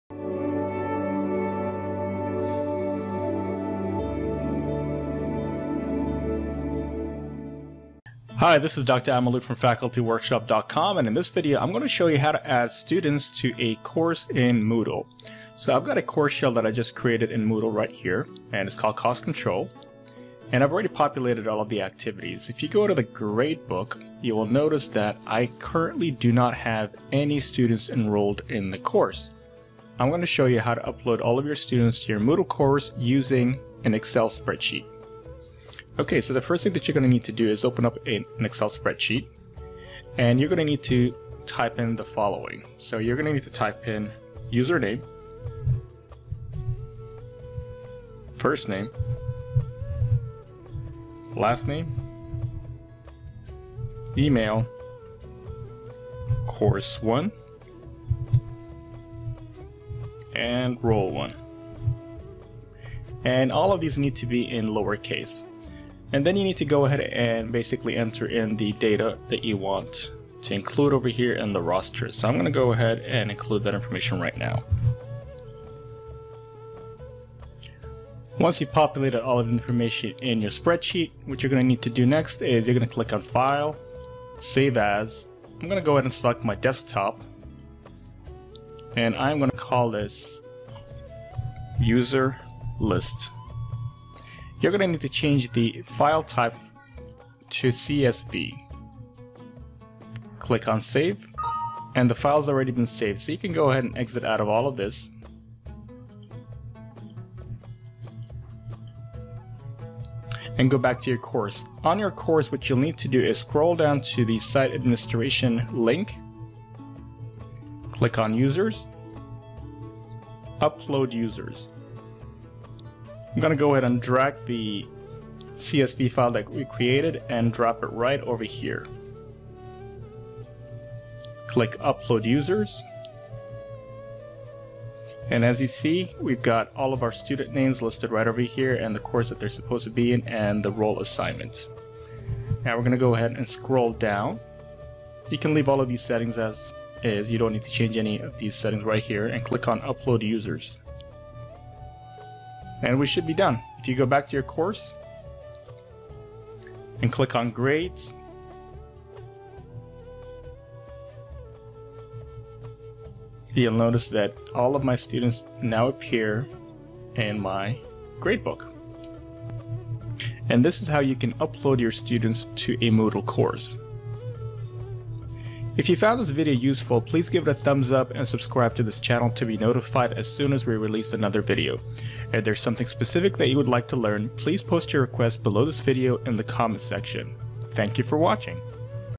How To Add Students To A Moodle Course
In this video, I 'll show you how to add students to a course in moodle from an excel (csv) spreadsheet.

STEPS:
1. Open Excel.
1. Create a spreadsheet with the usernames, first name, last name, emails of the students/
2. Save the spreadsheet as a csv file
3. Go to Moodle.
4. Click on Site Administration.
5. Click on Users.
6. Click Upload Users.
7. Drag the csv file to the drop inbox.
8. Click upload users

If you have any special tutorial requests, please post them below this video.